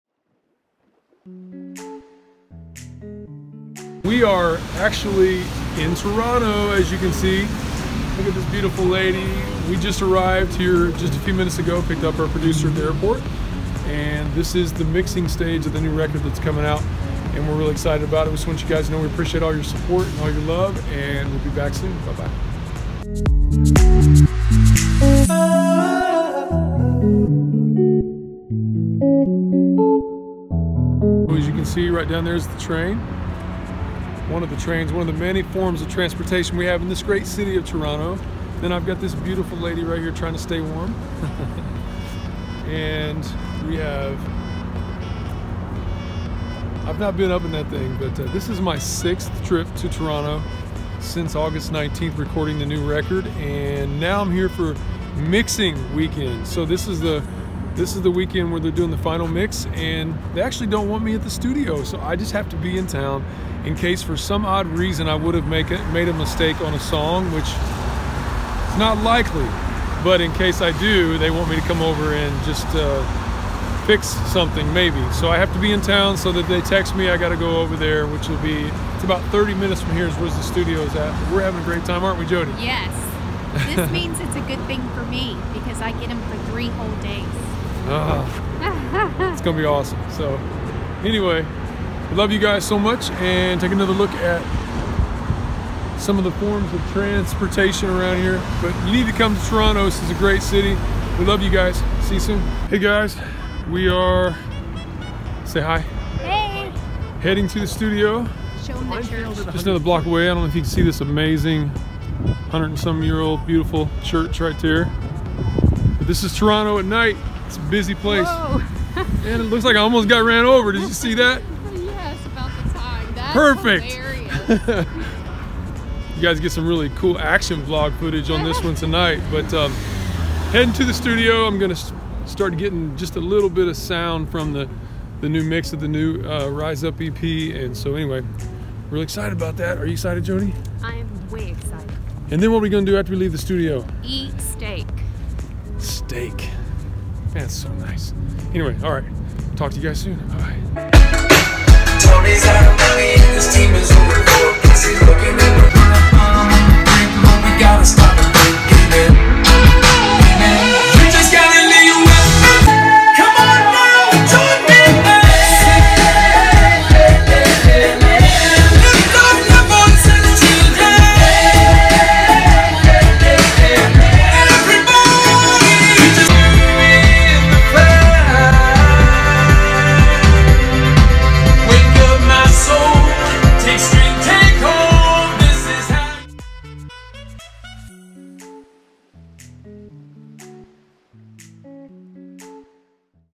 Day In The Life | New Music, Toronto, Almost Got Hit By A Car | Billy Ballenger
March with me to Winter Jam 2020 as you watch my weekly vlogs along the way.